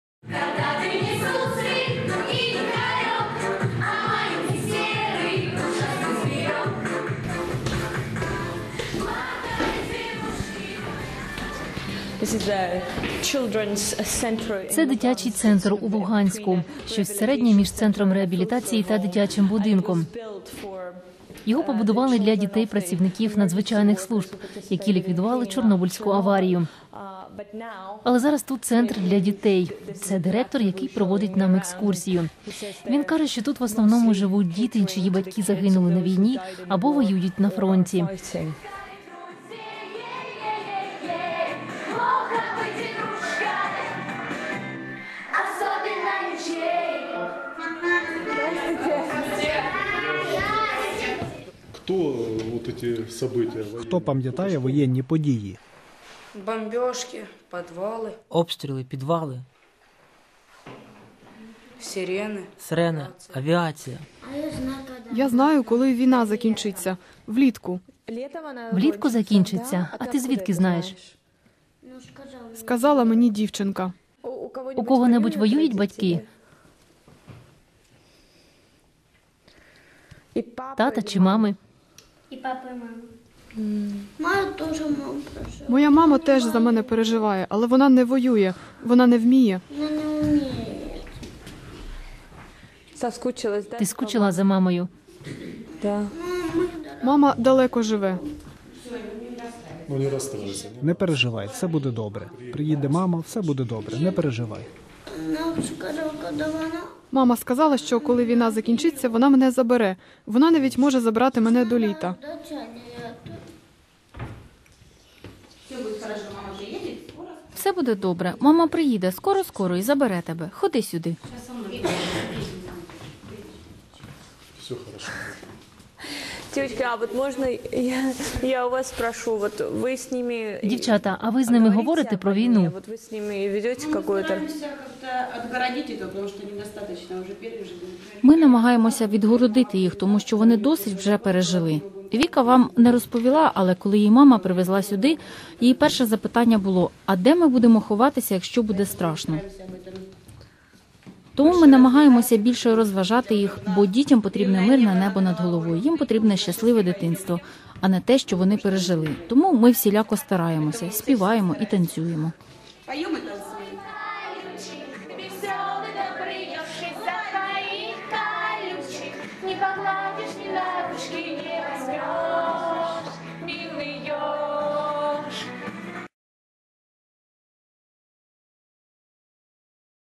Обличчя конфлікту на Сході: діти війни
Кореспонденти ВВС відвідали дитячий центр у Луганську, де живуть дітлахи, чиї батьки пішли на фронт, або ж загинули на війні.